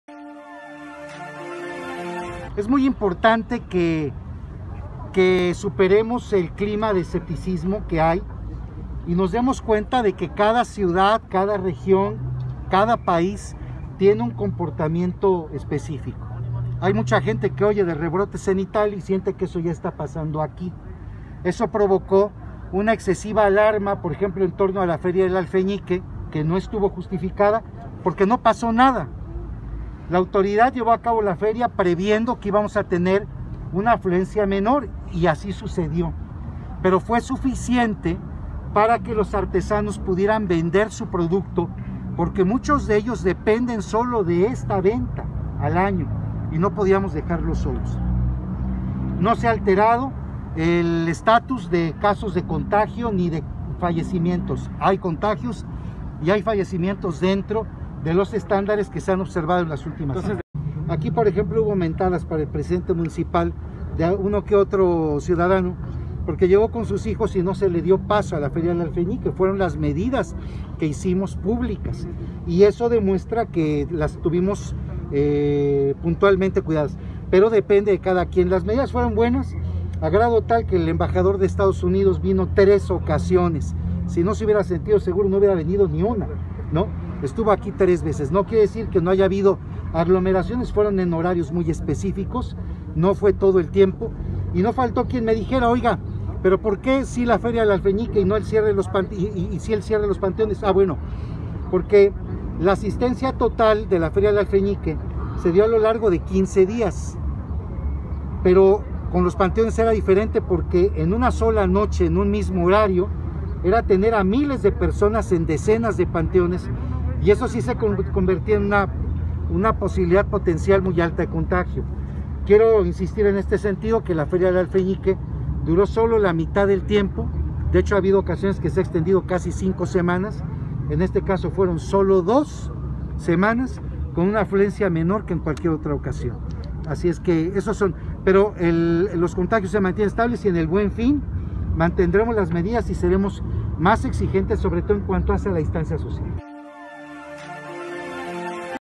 Juan Rodolfo Sánchez
¿Por qué sí se hizo la Feria del Alfeñique y por qué no se abrieron los panteones en festividad de DíadeMuertos en Toluca? Juan Rodolfo Sánchez